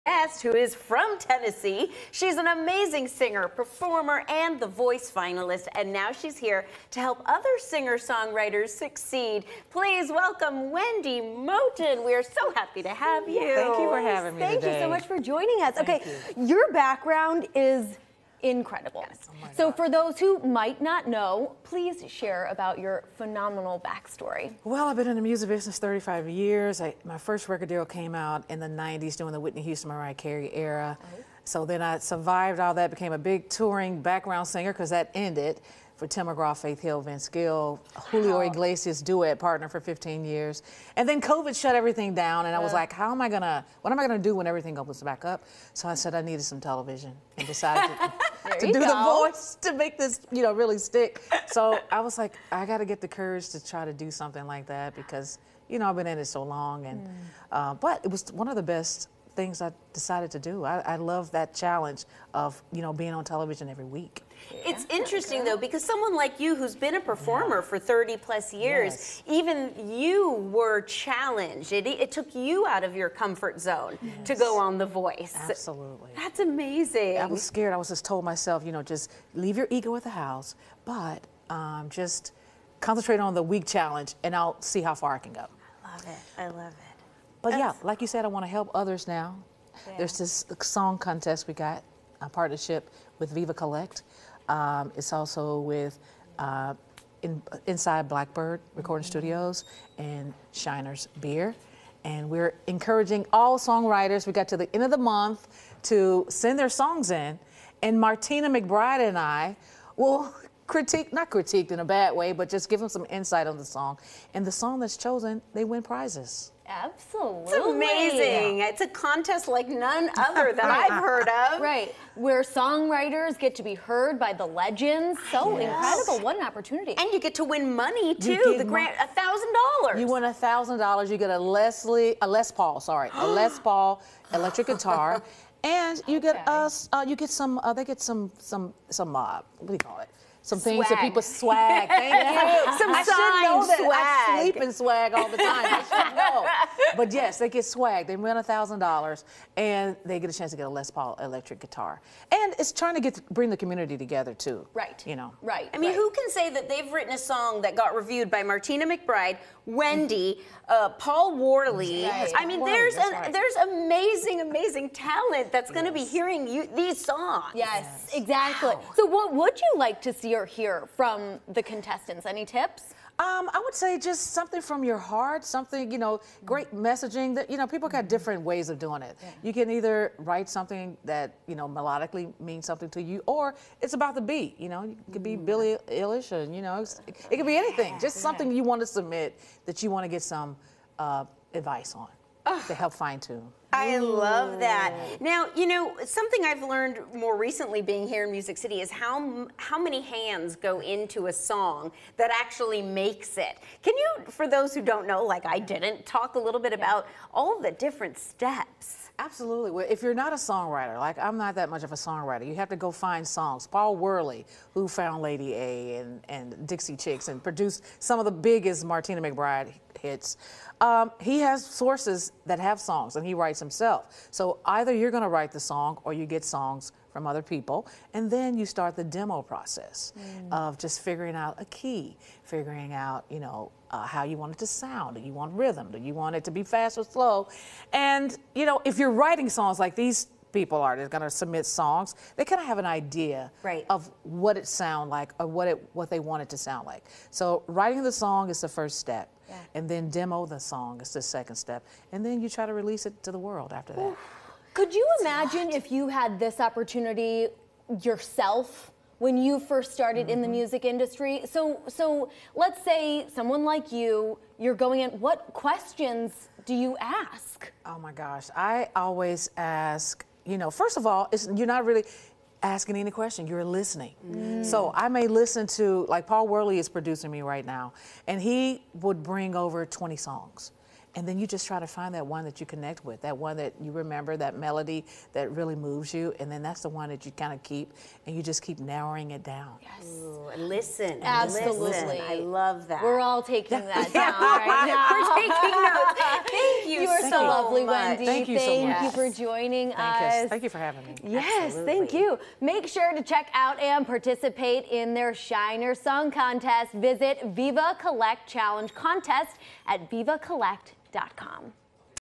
Shiner Song Contest
The Voice finalist Wendy Moten talks about creating and reviewing songs with Martina McBride and Paul Worley.